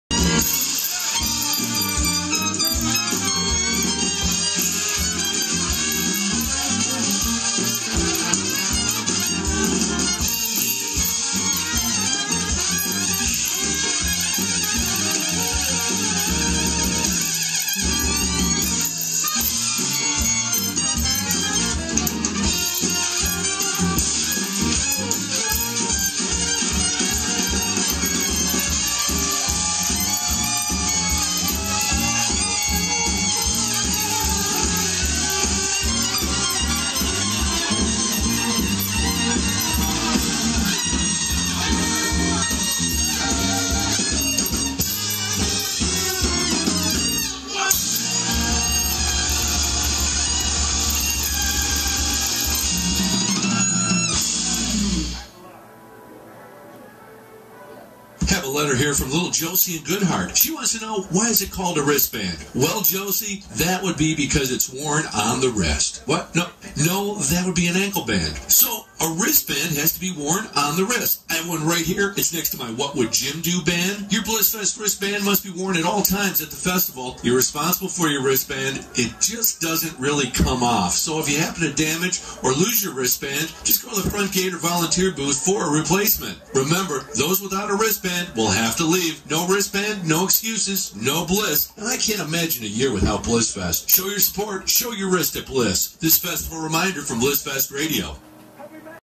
blissfest radio is a internet radio station worth checking out. If you like Bluegeass, Folk, maybe a little Cajun? or a bit of Rock n Roll, stop on by cause this is where it's at. Blissfest is a 3 day Family oriented Concert, held every year in Northern Michigan. They are celabrating their 31'st year. So what are you waiting for? See ya there. Turn it up!